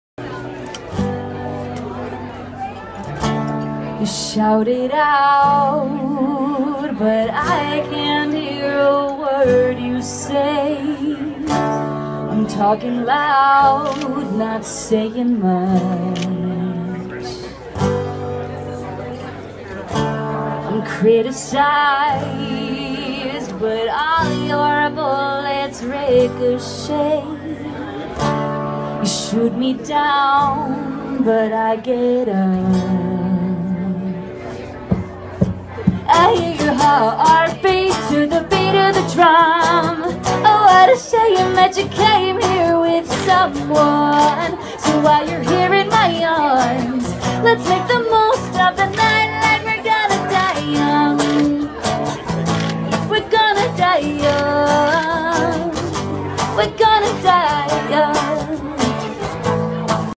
Mockingbird performs at Lebanon Family Health Services Splash of Art Party and Auction
By: Jeremy Long
Published on: April 12, 2015